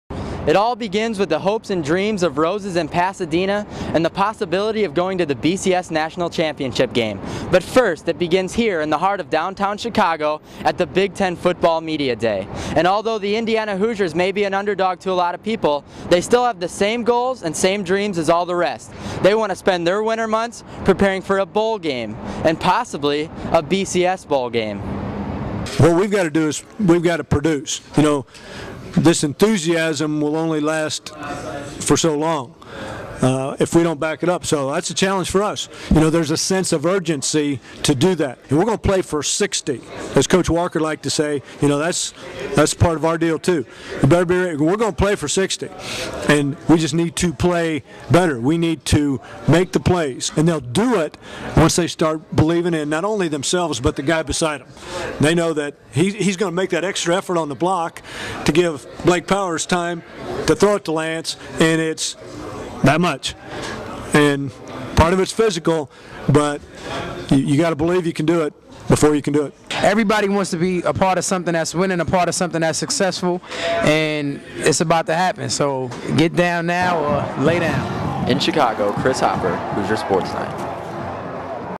Big Ten Football Media Day
This is a package I put together from the Big Ten Media Day in Chicago.